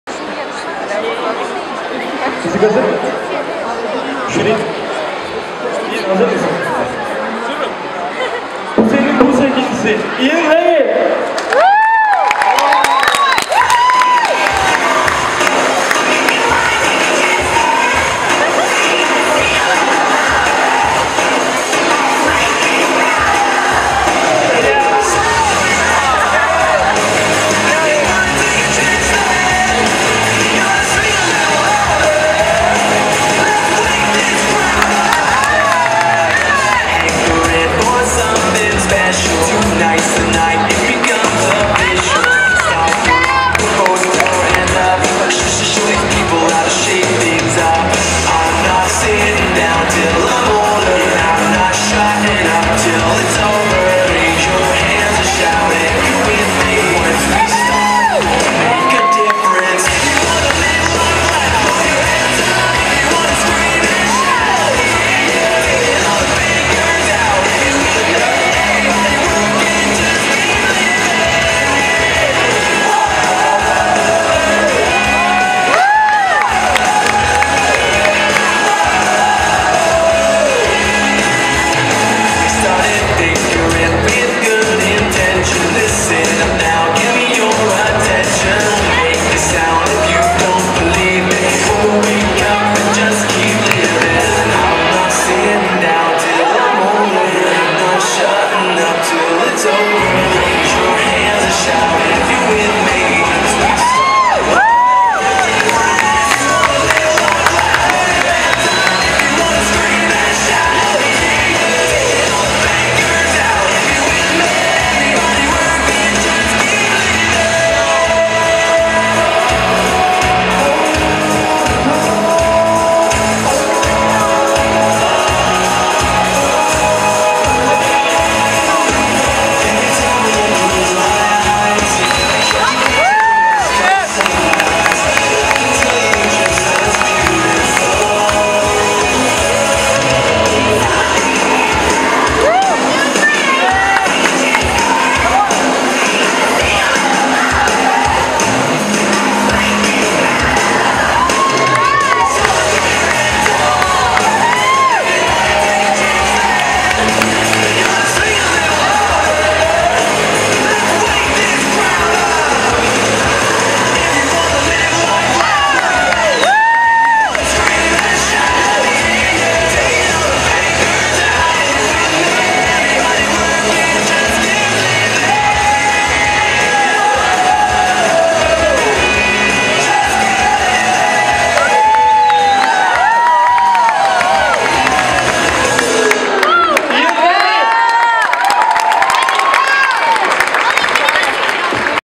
11th place Ian Lammi 2012 TYYC
My routine for the 2012 TYYC Finals which earned my 11th place